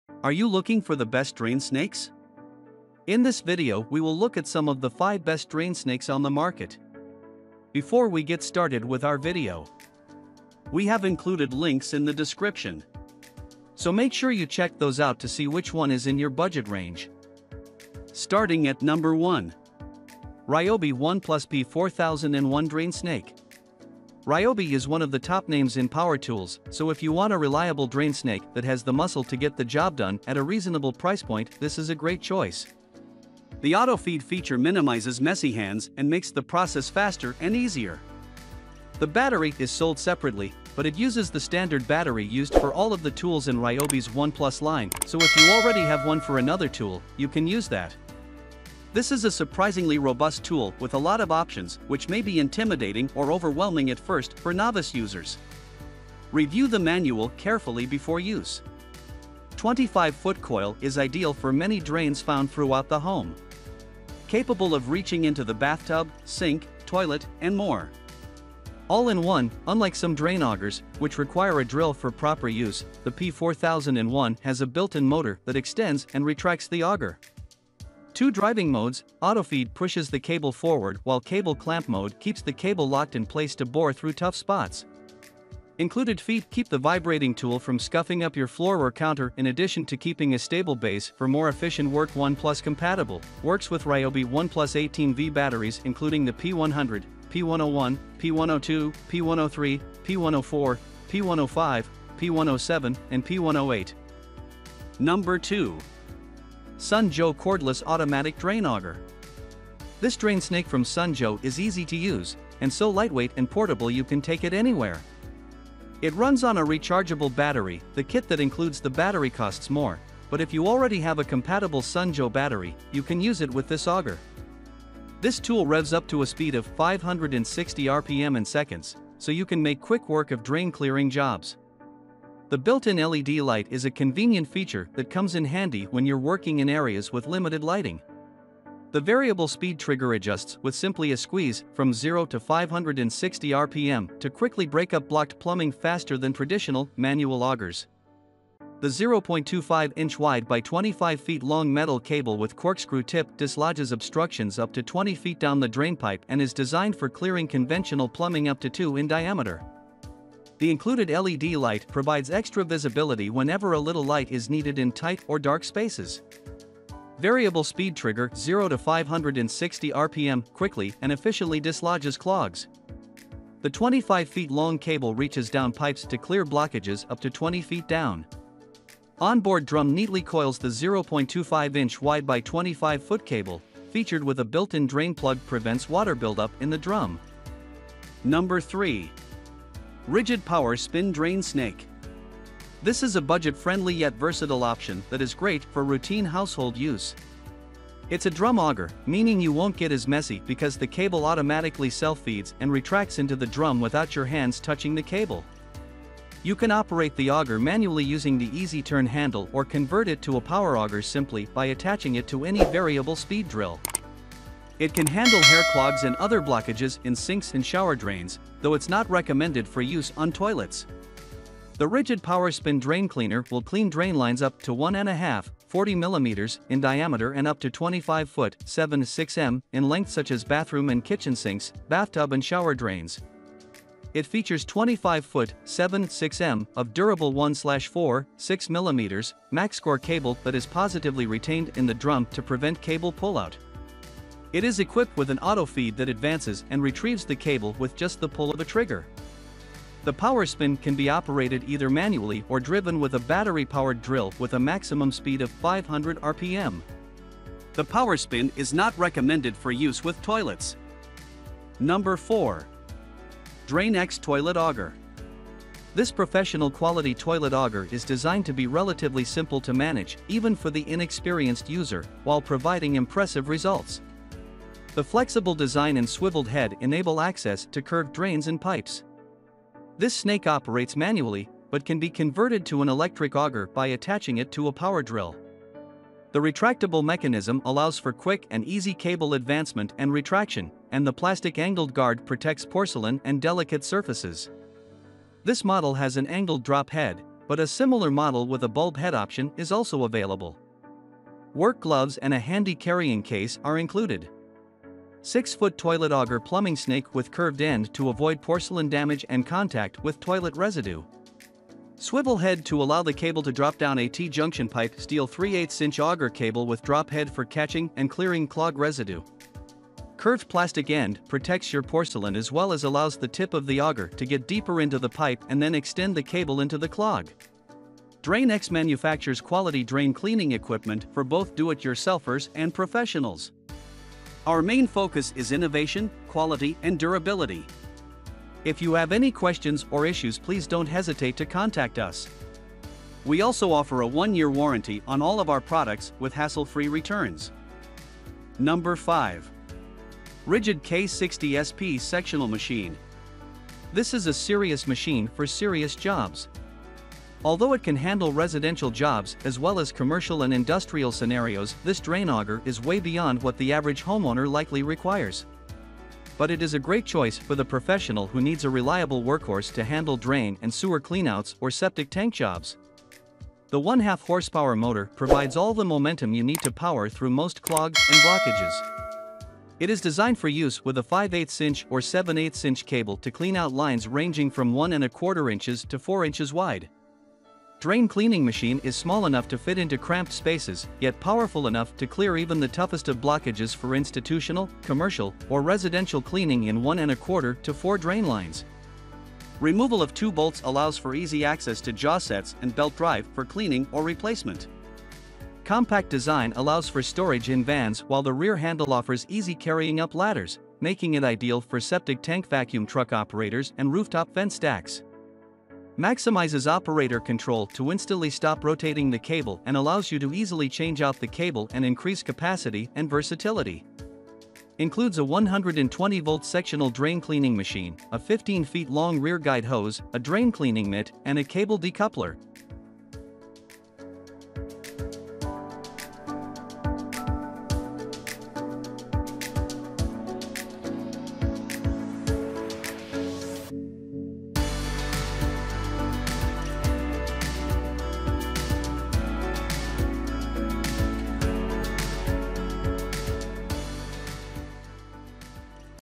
Best Drain Snakes in 2023 || Top 5 Best Drain Snake You can Buy Review
📌Product Link📌:
1. Ryobi ONE+ P4001 Drain Snake.

2. Sun Joe Cordless Automatic Drain Auger.

3. Ridgid Power Spin Drain Snake.

4. DrainX Toilet Auger.

5. Ridgid K-60SP Sectional Machine.

In this video, we listed the Best Drain Snakes in 2023 ?

1. Ryobi ONE+ P4001 Drain Snake.
2. Sun Joe Cordless Automatic Drain Auger.
3. Ridgid Power Spin Drain Snake.
4. DrainX Toilet Auger.
5. Ridgid K-60SP Sectional Machine.